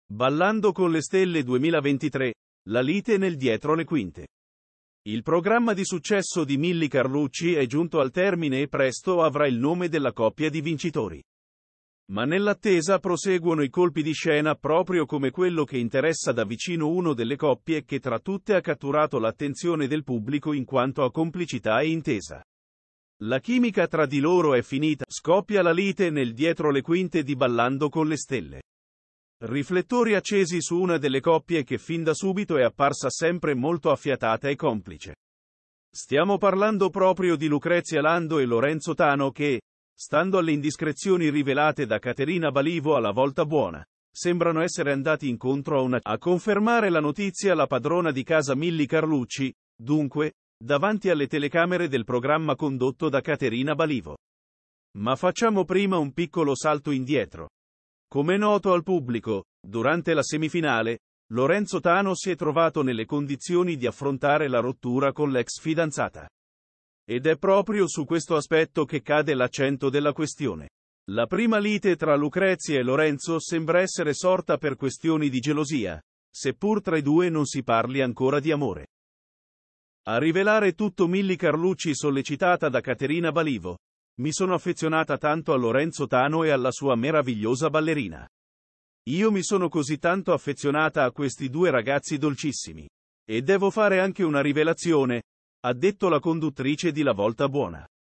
“Li hanno visti dietro le quinte”. Lorenzo Tano e Lucrezia, la scoperta a Ballando con le stelle.
Ballando con le stelle 2023, la lite nel dietro le quinte. Il programma di successo di Milly Carlucci è giunto al termine e presto avrà il nome della coppia di vincitori